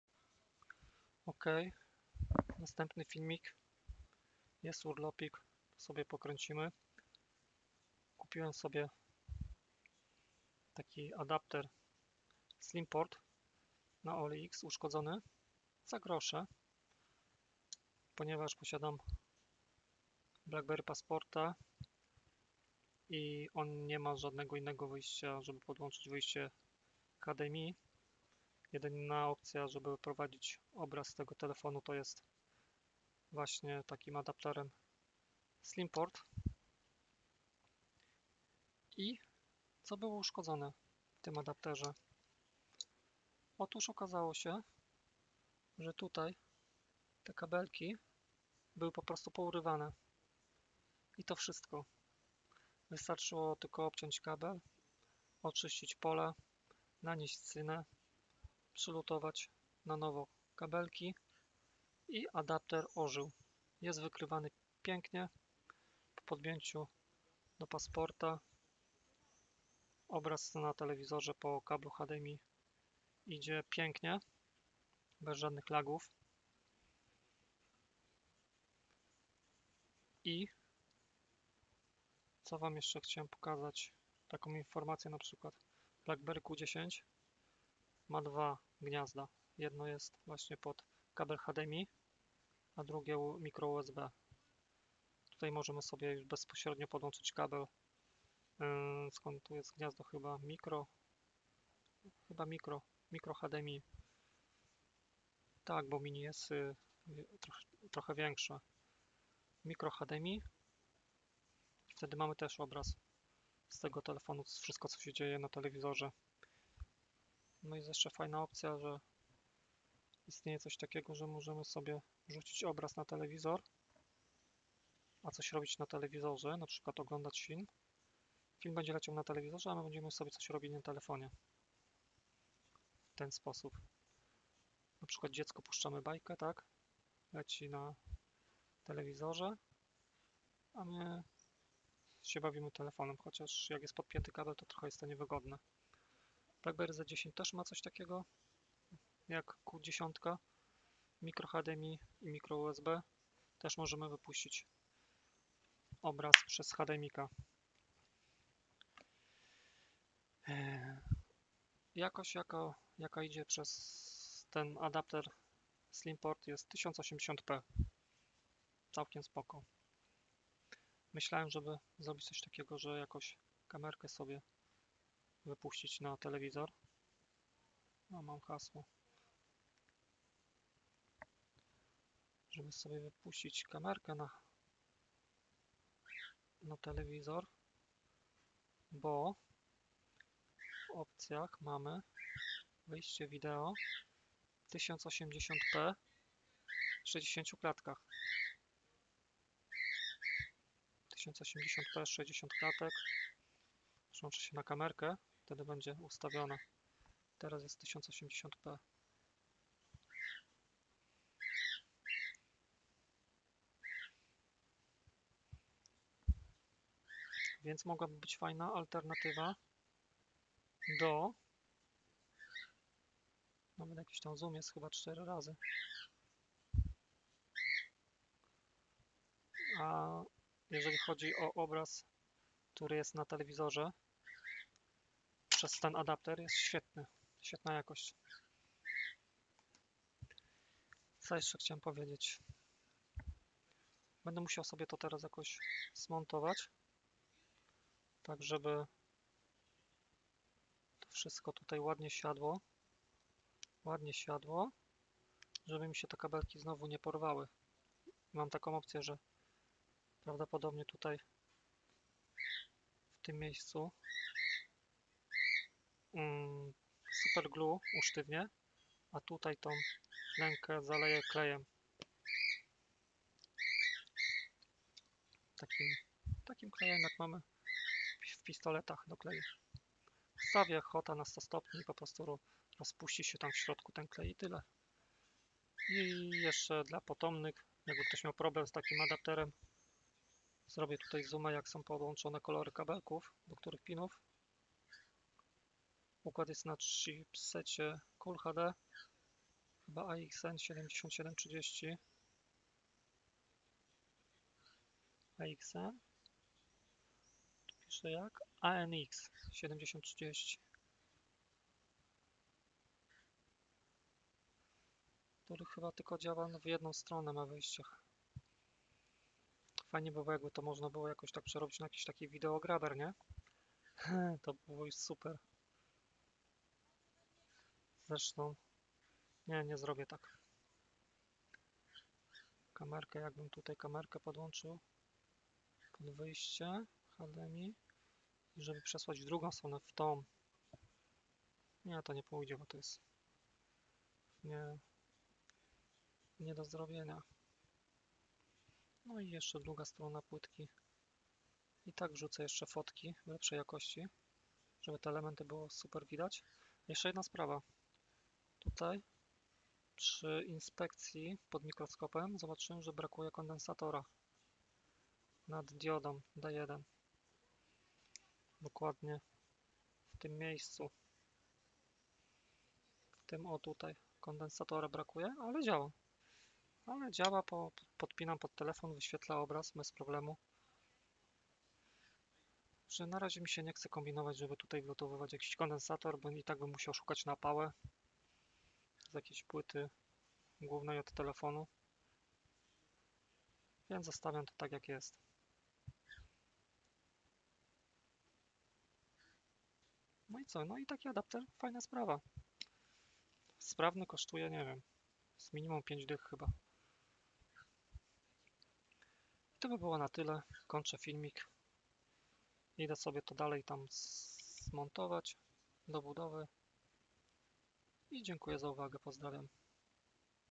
Adapter Slimport FullHD 1080P
Kupiłem na olx uszkodzony adapter Slimport. Potrzebowałem go do podłączenia BlackBerry Passporta przez HDMI do tv 4K.

Na szczęście udało się naprawić. Były tylko oderwane kabelki od płytki. Po przylutowaniu wszystko działa elegancko